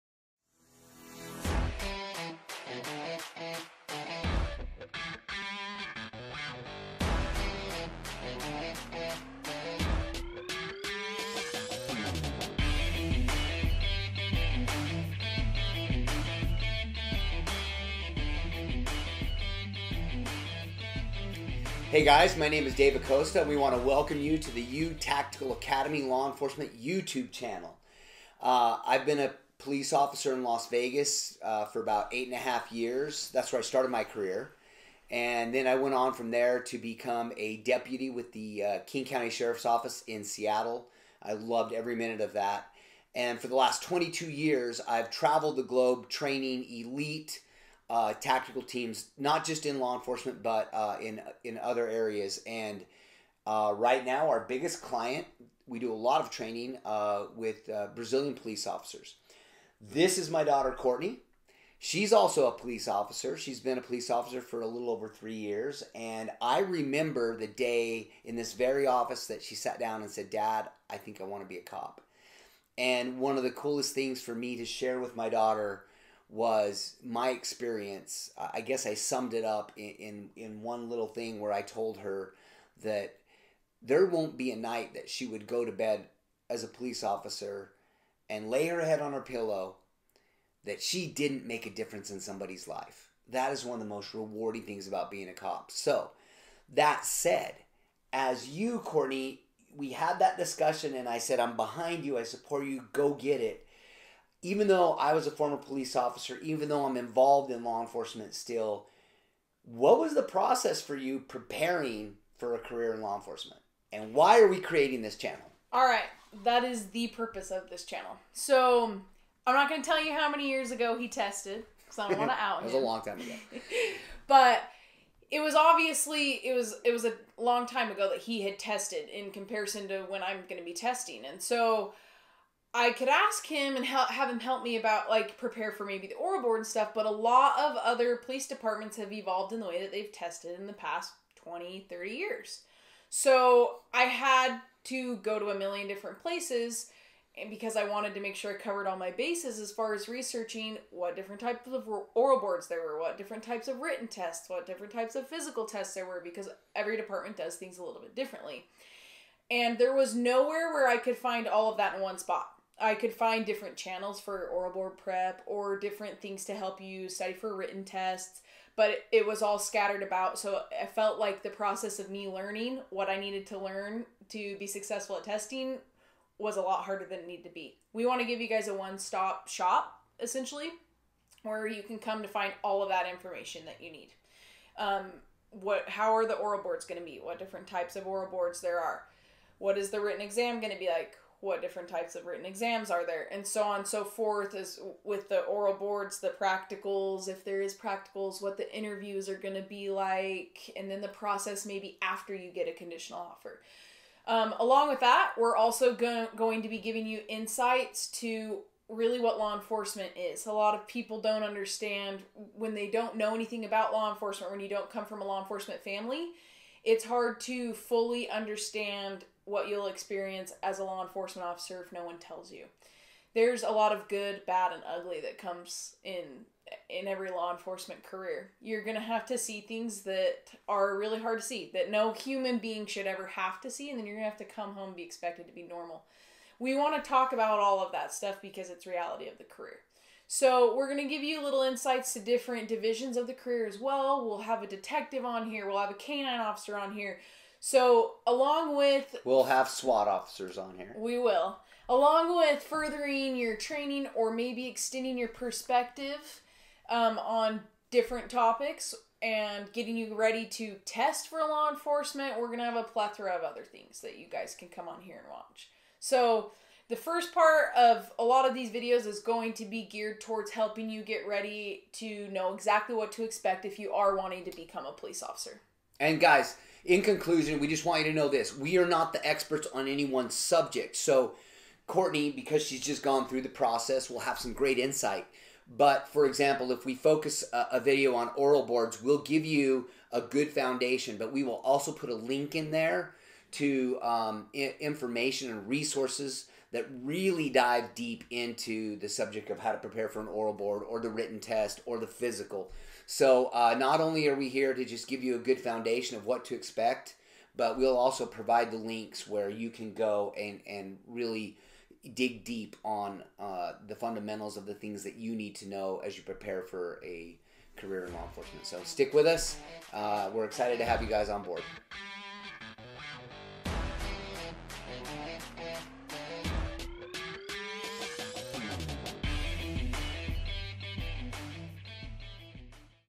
Channel Description - YouTactical Academy LE
We are excited to share information with those of you considering a career in Law Enforcement.  This video gives you a quick overview of what type of content we will be producing for this channel.

Upcoming topics (future videos) will include:

Written Test - Prep Resources
Oral Boards - Prep & Resources
Fitness Tests - Prep
Practical Exams
We will also address general/common questions about law enforcement via interviews with Detectives, SWAT, K9, Intel Unit, Narcs and many other law enforcement specialties.